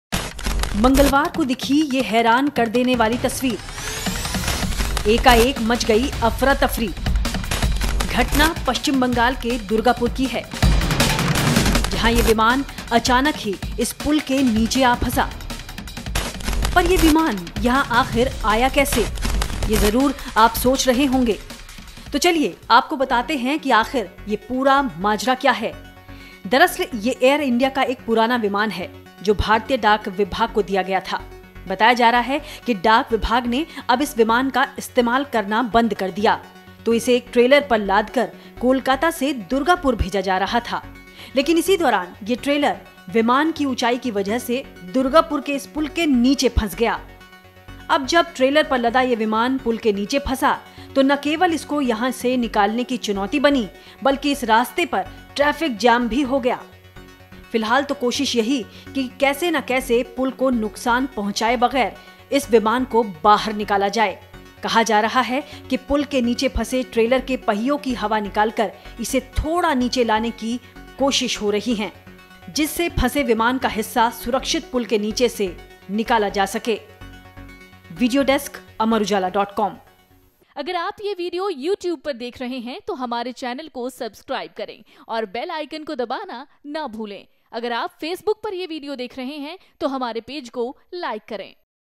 West Bengal के Durgapur में पुल के नीचे फंसा Aeroplane, मची अफरा-तफरी | Aeroplane Stuck Under Bridge
West Bengal के जिले दुर्गापुर में मंगलवार को Indian Postal Department का एक पुराना Plane एक पुल के नीचे फंस गया। बताया जा रहा है कि Air India के इस पुराने विमान को एक ट्रेलर पर लादकर Kolkata से Durgapur भेजा गया था। देखिए कैसे इस विमान को पुल के नीचे से निकालने की कोशिशें की जा रही हैं।